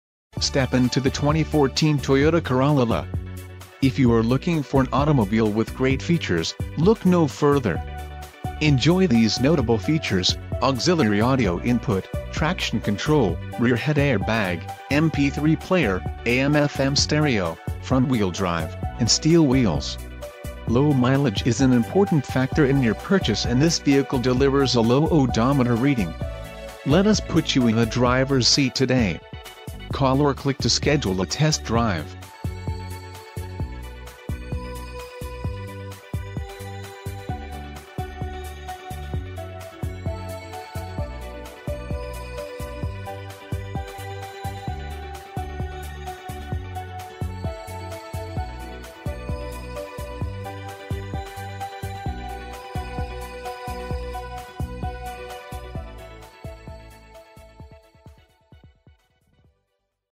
2014 Toyota Corolla LE
The best just got better! Our 2014 Toyota Corolla LE shown here in dynamic Super White is eager to please. Under the stylish hood of this peppy sedan is a 1.8 Liter I-4 cylinder that offers 132hp on demand, while paired with a smooth CVT. This ideal combination helps you achieve near 38 mpg on the open road and is perfect for your commute. It's no surprise that our Corolla ranks as the best-selling car of all time! Now with a wider, more aggressive stance, the exterior of the LE is a perfect balance of style and efficiency. The attractive metallic- accented interior boasts even more legroom than before with technology that is straightforward and user-friendly. You'll appreciate key-less entry, heated mirrors, cruise control, and a 60/40-split-folding rear seat. A 6 inch touchscreen display acts as your personal command center, while Bluetooth, iPod connectivity and steering wheel mounted audio controls are also handy.